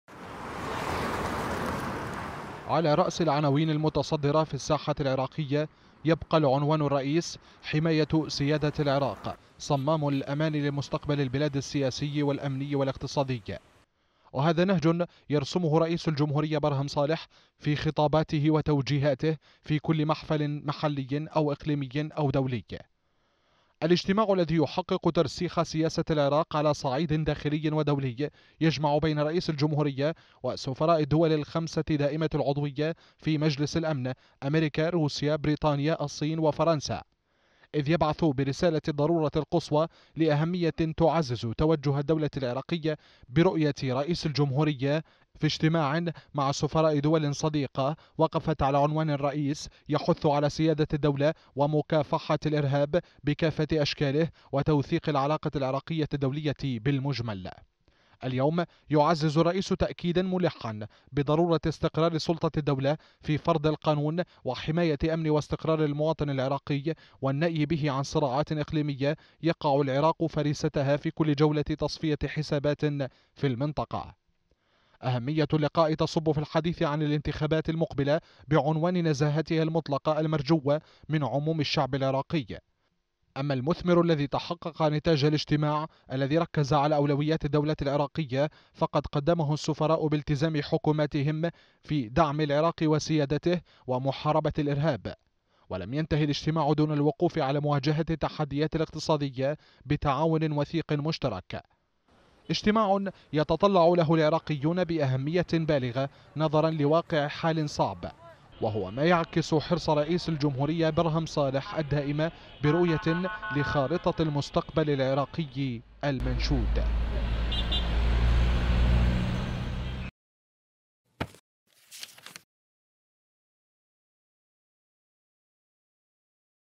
برهم صالح يعيد العراق إلى الواجهة الدولية والسيادة هي الاولوية | تقرير
بهدف النأي بالعراق وشعبه عن أية صراعات إقليمية تقع البلاد فريستها في كل جولة تصفية حسابات في المنطقة، يرسخ اجتماع رئيس الجمهورية برهم صالح بسفراء الدول دائمة العضوية على ثوابت راسخة تحفظ سيادة العراق وتوثق العلاقات المشتركة الفاعلة مع الدول العظمى.